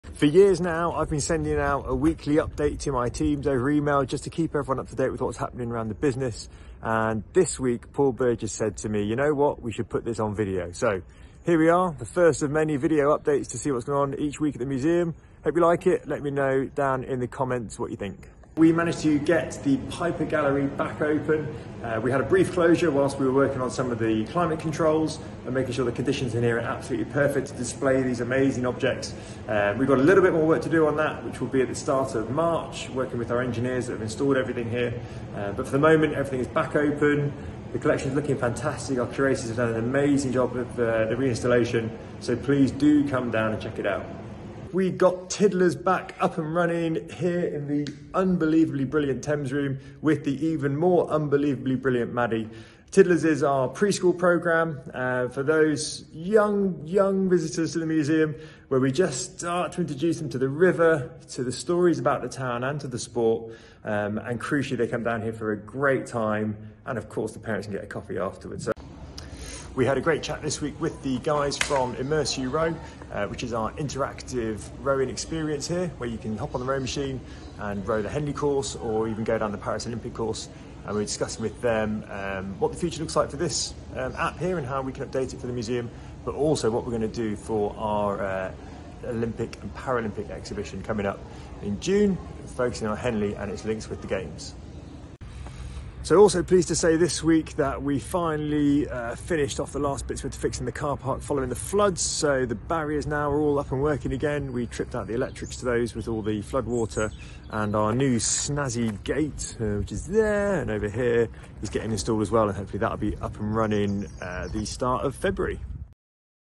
Director's update - 31st January 2024
Director Steve O'Connor shares the latest updates from the Museum, including the Piper Gallery, Tiddlers, immersive rowing and the car park!